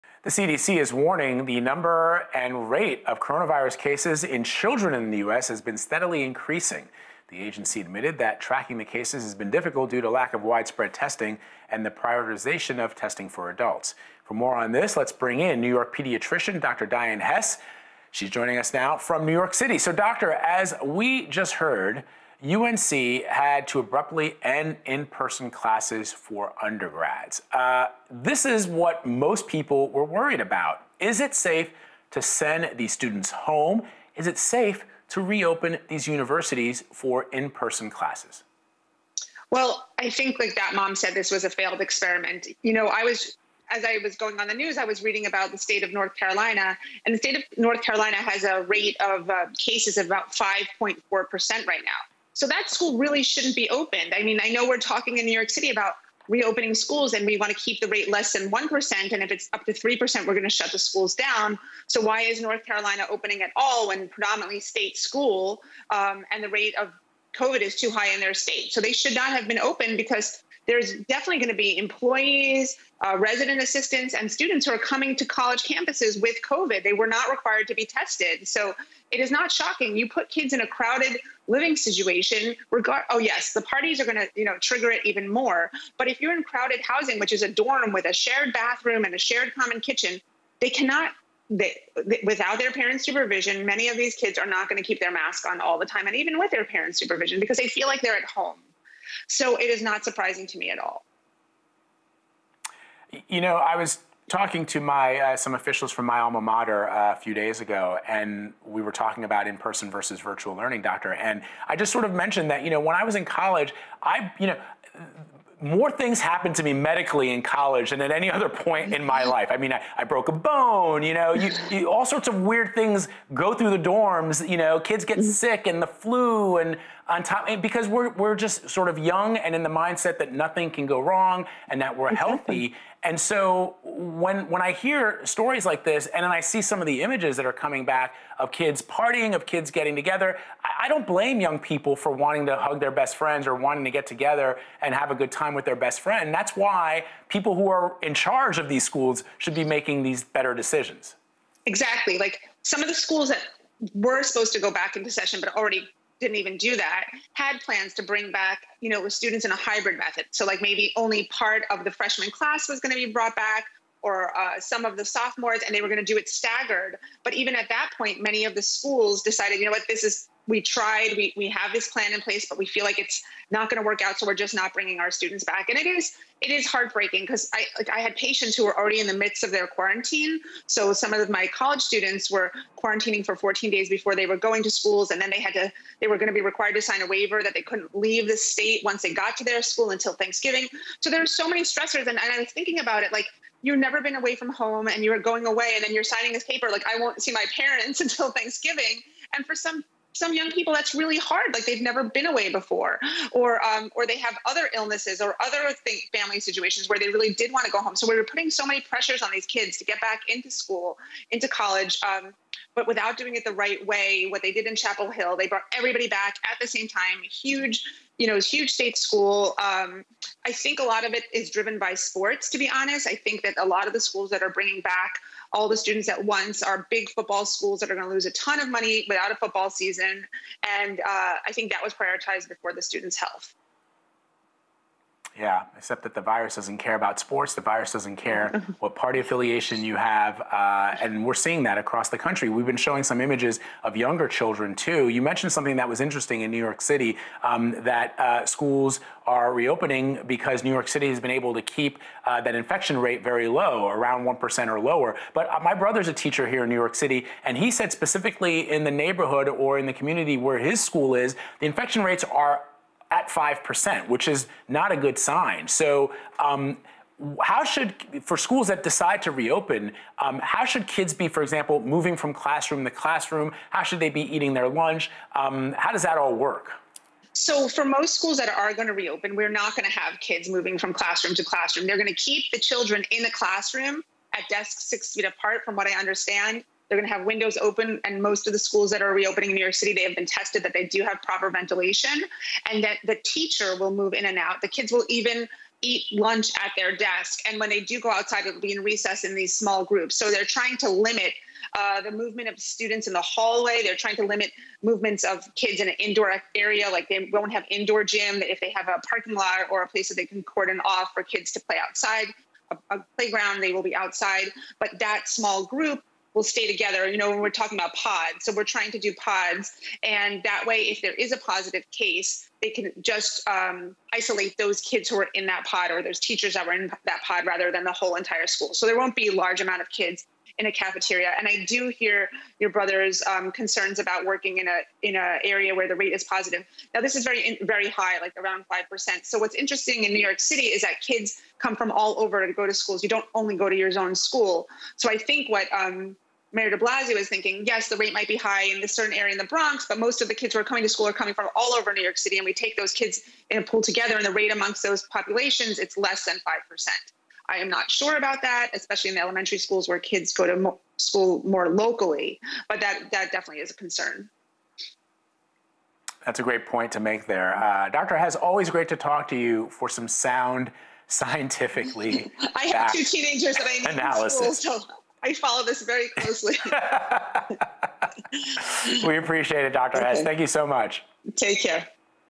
Schools struggle with reopening amid COVID-19 pandemic
As schools across the U.S. continue to struggle with how or whether to return to in-person learning, one big university is reversing course after more than 130 students tested positive for COVID-19. WNCN-TV's Emani Payne reports from North Carolina, and Dr. Dyan Hes joins CBSN to discuss the risks.